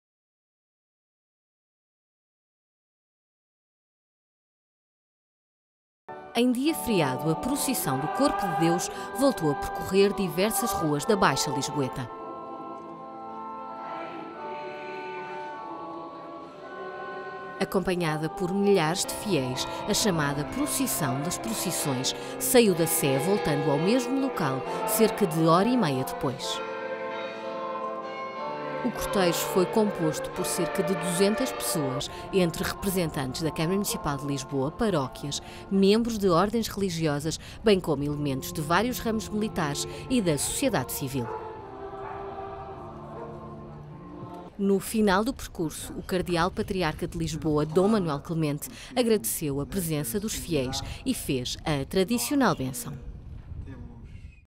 Procissão do Corpo de Deus
Decorreu, no dia 26 de maio, a Solene Procissão do Corpo de Deus, que percorreu diversas ruas da Baixa lisboeta, integrada e acompanhada por milhares de fiéis.